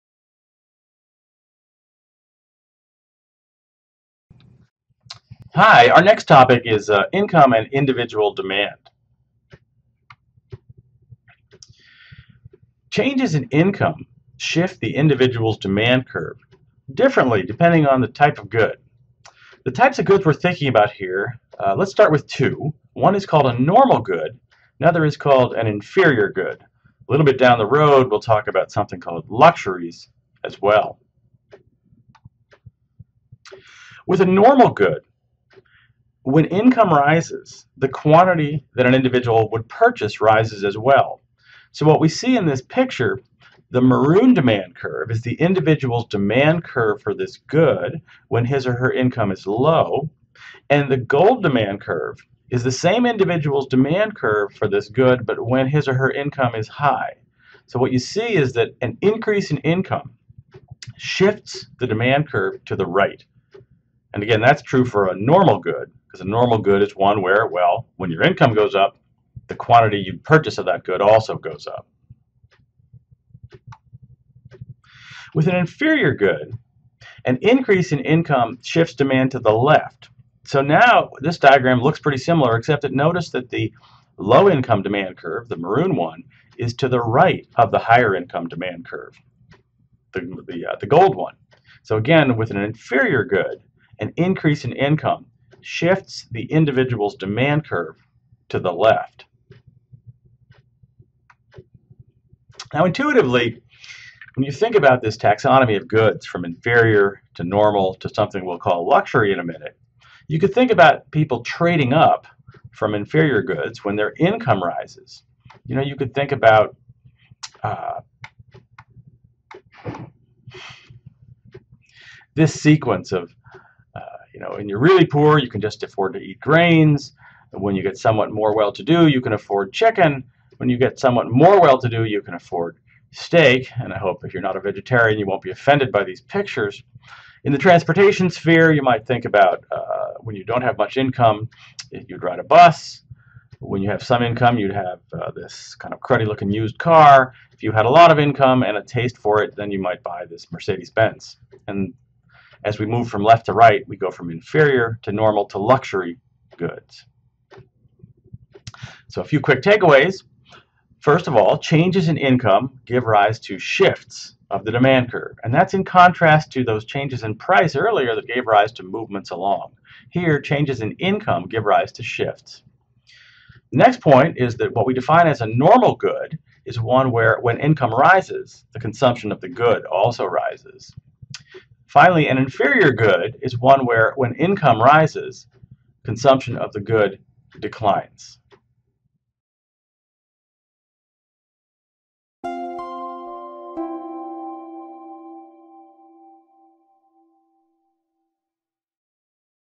Income and individual demand
This module continues our discussion of preferences with marginal utility and the MRS.

After this sub-module, you should know:
   * MRS is related to marginal utilities of the two goods
   * MRS=MUX/MUY
   * Marginal utility generally declines with consumption
   * Shape of indifference curves indicates willingness to substitute